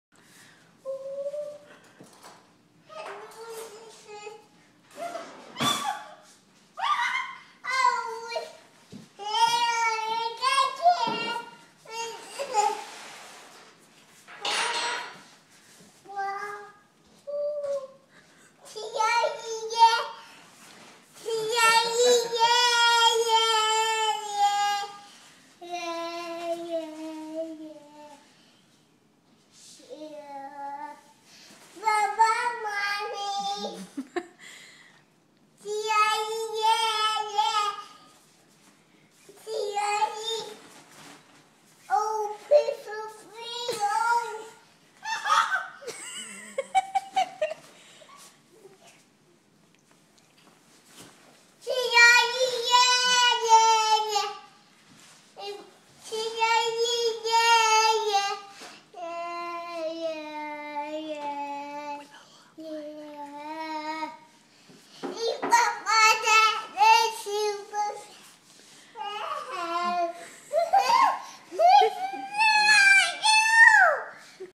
Puppy
Max singing in his puppy tent